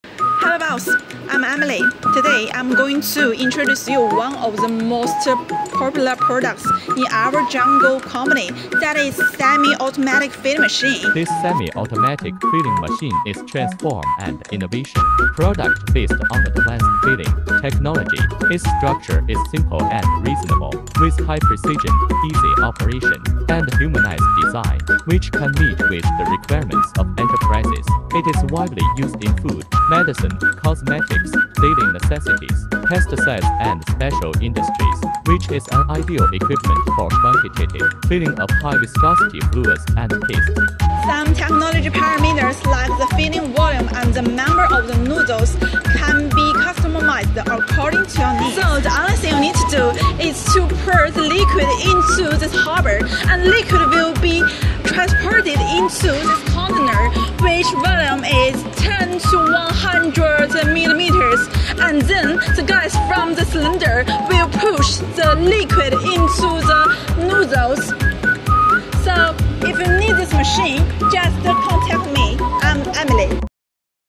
semi automatic filling machine from Joygoal
HIGH-PRECISION ANTI-DRIP FILLING SYSTEM
Use stainless steel material, rigorous manufacturing processAnti-drip system composed of multiple structuresFilling without dripping, more accurate quantification